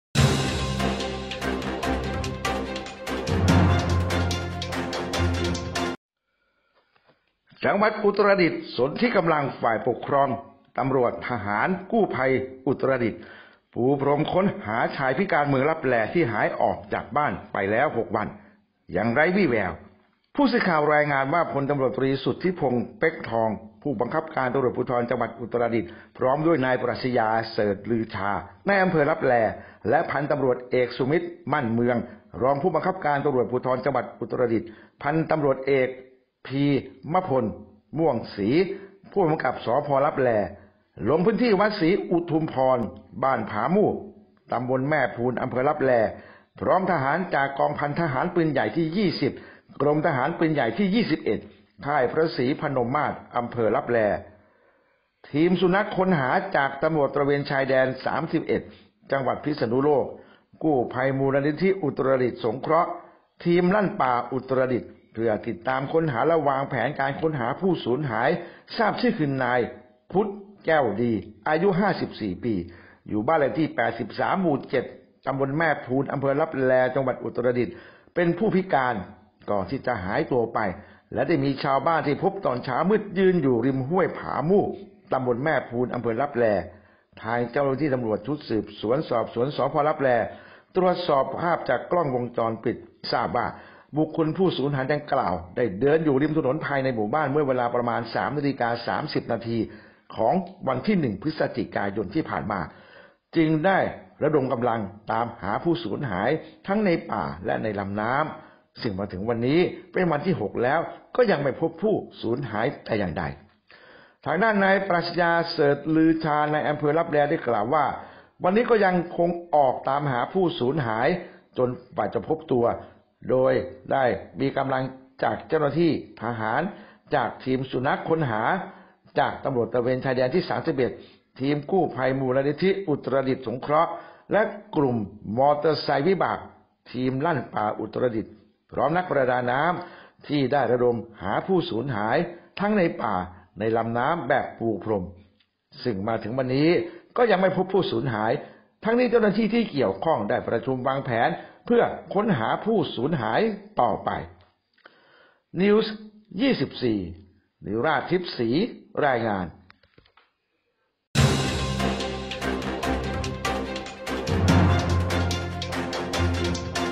ผู้บังคับการตำรวจภูธรจังหวัดอุตรดิตถ์นายอำเภอลับแลและผู้กำกับสถานีตำรวจภูธรอำเภอลับแลลงพื้น
ผู้บังคับการตำรวจภูธรจังหวัดอุตรดิตถ์นายอำเภอลับแลและผู้กำกับสถานีตำรวจภูธรอำเภอลับแลลงพื้นที่บ้านผามูบร่วมวางแผนติดตามผู้สูญหาย              เมื่อวันที่ 5 พฤศจิกายน 2564 เวลา 11.00 น
พลตำรวจตรีสุทธิพงศ์ เป๊กทอง ผู้บังคับการตำรวจภูธรจังหวัดอุตรดิตถ์และพ.ต.อ.ภีมภณ ม่วงศรี ผู้กำกับการสถานีตำรวจภูธรลับแล เจ้าหน้าที่ทหารจาก ป.พัน 20  ทีมกู้ภัยจังหวัดอุตรดิตถ์ กลุ่มมอเตอร์ไซด์วิบากทีมลั่นป่าอุตรดิตถ์ ลงพื้นที่วัดศรีอุทุมพร ตำบลแม่พูล เพื่อให้กำลังใจเจ้าหน้าที่ตำรวจ เจ้าหน้าที่กู้ภัย นักประดาน้ำ ที่ระดมหาผู้สูญหาย คือนายพุทธ แก้วดี อายุ 54 ปี ซึ่งทราบจากบุคคลในครอบครัวว่าได้หายออกจากบ้านไปตั้งแต่เช้าตรู่วันที่ 1 พฤศจิกายนที่ผ่านมา โดยชาวบ้านที่พบเห็นแจ้งว่า พบผู้สูญหายยืนอยู่ริมห้วยผามูบและทางเจ้าหน้าที่สืบสวนตำรวจ สภ.ลับแลได้พบภาพจากกล้องวงจรปิดว่าบุคคลผู้สูญหายได้เดินอยู่ริมถนนภายในหมู่บ้านในเวลา 03.30 น.ของวันที่ 1  จึงได้ระดมกำลังตามหาผู้เสียหาย ทั้งในป่า-และในลำน้ำ  ซึ่งมาถึงวันนี้ก็ยังไม่พบผู้สูญหายแต่อย่างใด ทั้งนี้เจ้าหน้าที่ที่เกี่ยวข้องได้ประชุมวางแผนเพื่อค้นหาผู้สูญหาย
     และต่อมาในเวลา 17.00 น.ของวันนี้ นายปรัชญา เสริฐลือชา นายอำเภอลับแล ได้ลงพื้นที่วัดศรีอุทุมพร ตำบลแม่พูล โดยมี พ.ต.อ. สุมิตร มั่นเมือง รอง ผบก.ภ.จว. อุตรดิตถ์พ.ต.อ.ภีมภณ ม่วงศรี ผู้กำกับการสถานีตำรวจภูธรลับแล เจ้าหน้าที่ทหารจาก ป.พัน 20 ทีมสุนัขค้นหา จาก ตชด.31 ทีมกู้ภัยจังหวัดอุตรดิตถ์ กลุ่มมอเตอร์ไซด์วิบากทีมลั่นป่าอุตรดิตถ์
     โดยได้ให้กำลังใจแก่ทีมเจ้าหน้าที่ค้นหาและมอบน้ำดื่มสะอาดให้แก่ทีมเจ้าหน้าที่              ด้านพ.ต.อ.ภีมภณ ม่วงศรี ผู้กำกับการสถานีตำรวลจภูธรลับแล ได้แจ้งแก่ทีมค้นหาว่าในวันพรุ่งนี้วันที่ 6 พฤศจิกายน จะเริ่มปฎิบัติการค้นหาผู้สูญหายอีกครั้ง ตั้งแต่เวลา 09.00 น เป็นต้นไป                       เอนก ธรรมใจ รายงาน